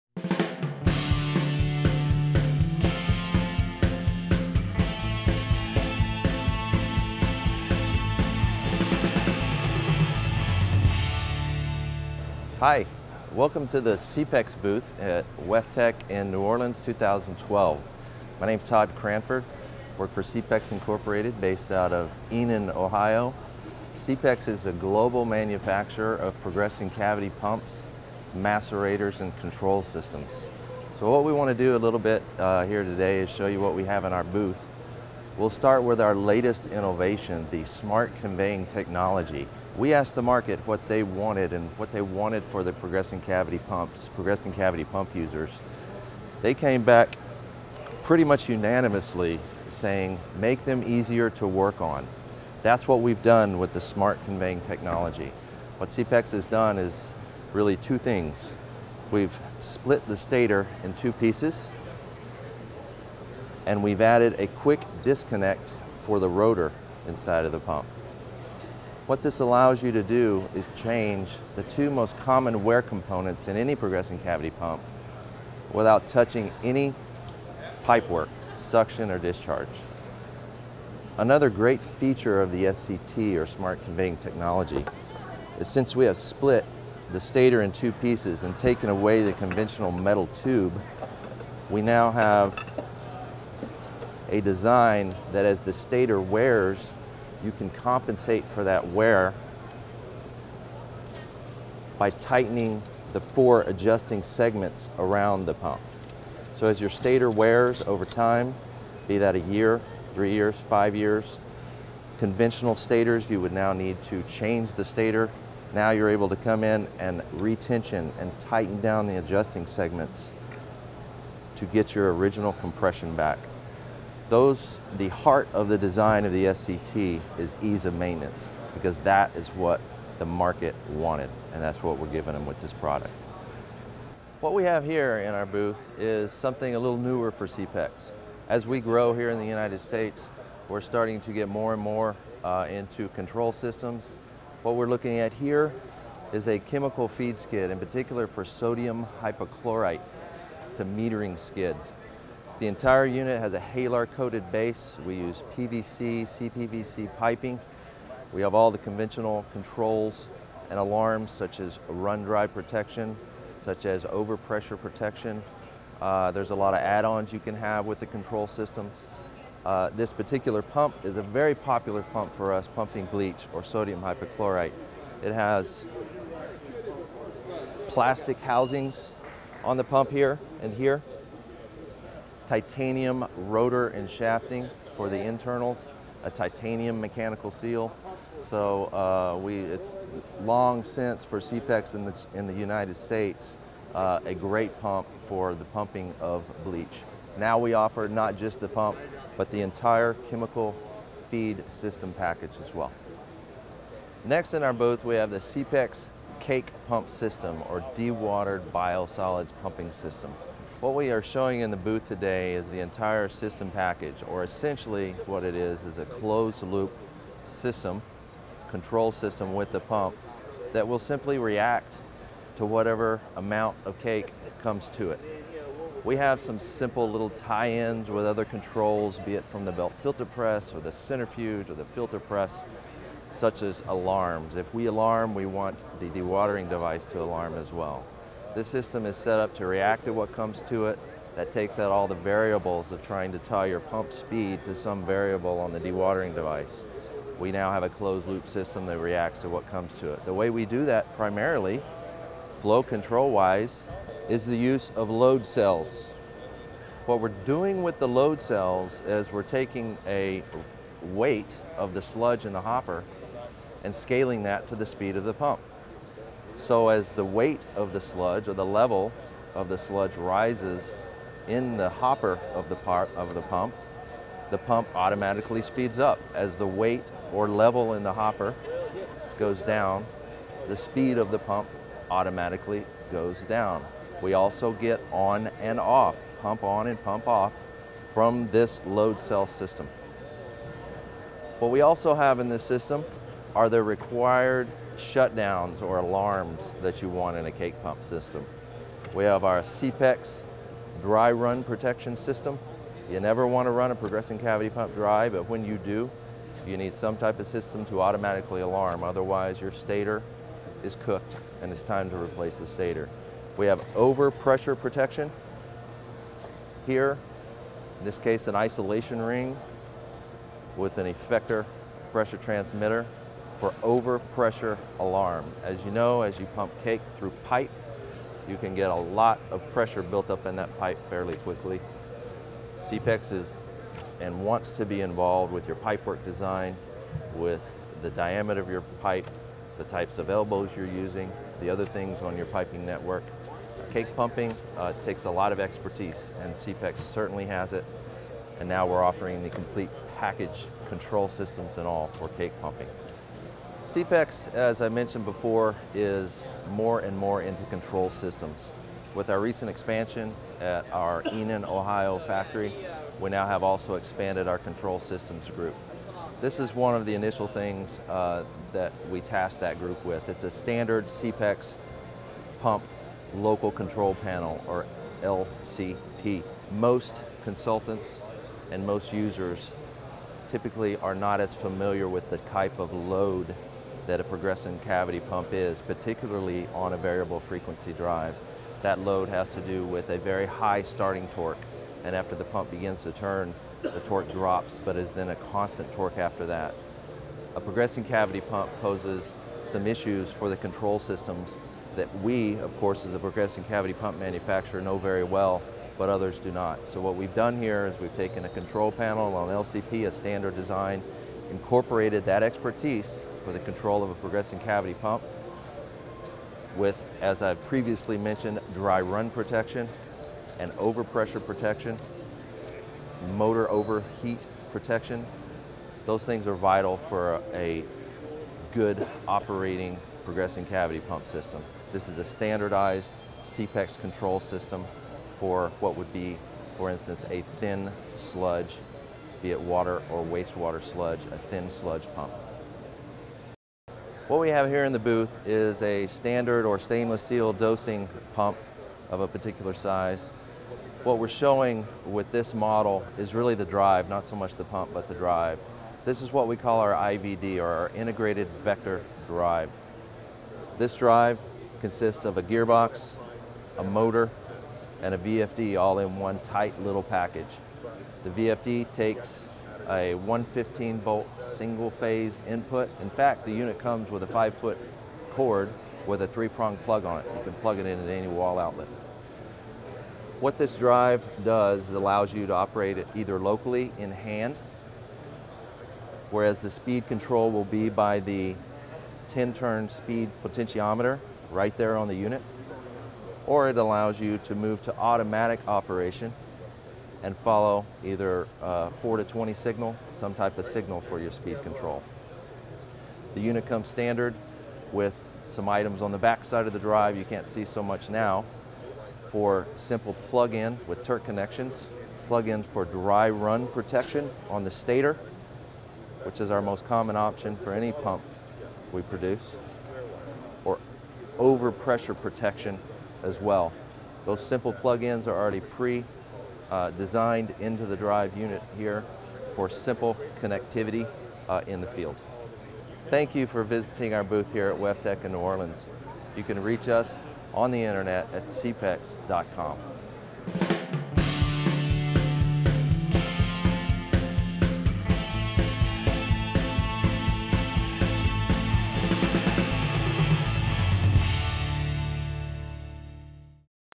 SEEPEX Fairs and Events: WEFTEC 2012
Todd Cranford, seepex Regional Sales Manager explains what was shown at the seepex booth at WEFTEC 2012 in New Orleans;  our award winning SCT, chemical metering skid system with controls, and Flow Control Innovation Award winning cake pump system.